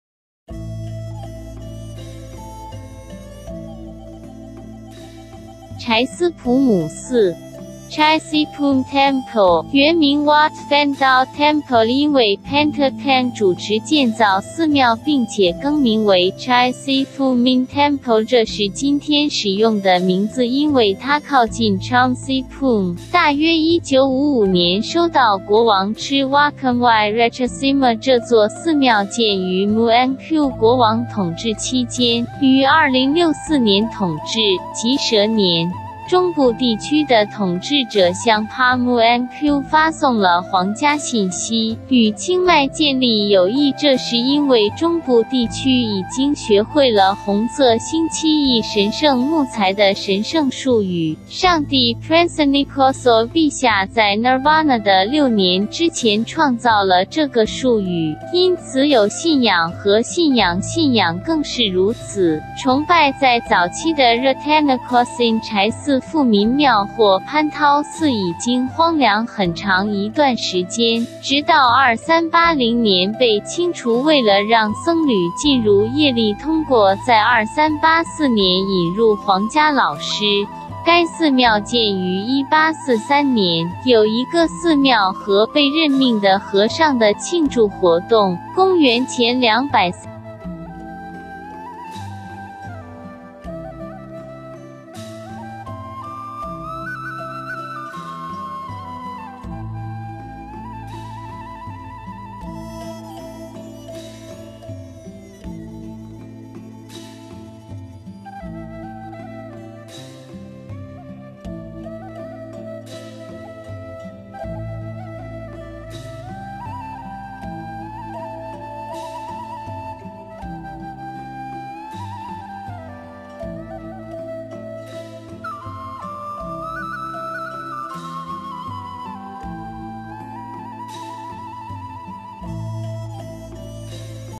29 วัดชัยศรีภูมิ ch
柴斯普姆寺
Chai Si Phum Temple，原名Wat Phan Tao Temple因为Panta Keng主持建造寺庙并且更名为Chai Si Phu Min Temple这是今天使用的名字
因为它靠近Chum Si Phum，大约1955年收到国王吃Wakamwai Ratchasima
这座寺庙建于Muang Kaew国王统治期间，于2064年统治，即蛇年。
中部地区的统治者向Phra Muang Kaew发送了皇家信息。与清迈建立友谊
这是因为中部地区已经学会了红色星期一神圣木材的神圣术语，上帝Prensenikosol陛下在Nirvana的6年之前创造了这个术语。
因此有信仰和信仰信仰更是如此。崇拜在早期的Rattanakosin柴寺富民庙或潘涛寺已经荒凉很长一段时间，直到2380年被清除为了让僧侣进入业力
通过在2384年引入皇家老师，该寺庙建于1843年。有一个寺庙和被任命的和尚的庆祝活动。
公元前238年，一位修道士建造了一座修道院。寺庙的重要性是建造成斯里兰卡城市的寺庙。曾经是一个木制的佛像红色陈核心王国Muangkaew的雕像